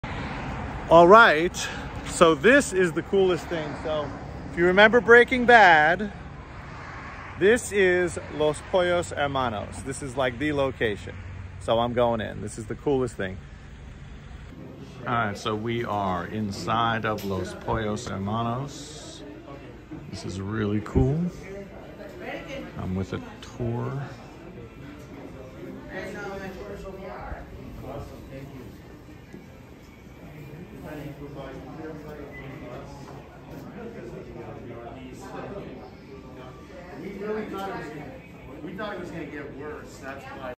Los Pollos Hermanos location. Breaking Bad!
This is the location where they filmed in Breaking Bad for Los Pollos Hermanos. It is an active fast food restaurant￼ for the chain Twisters.